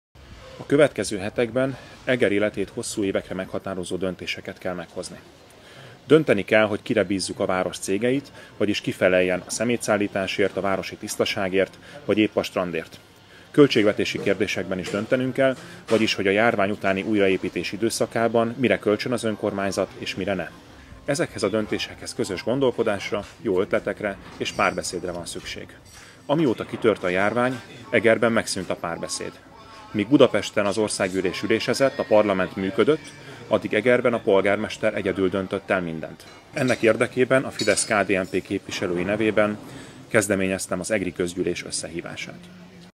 Az egri Fidesz-KDNP frakció kezdeményezte a városi közgyűlés összehívását
Eger életét hosszú évekre meghatározó döntéseket kell meghozni, az élet újraindításához közös gondolkodásra, párbeszédre van szükség. A Parlament is működik, ezért kezdeményezte a Fidesz-KDNP frakciója a városi közgyűlés összehívását.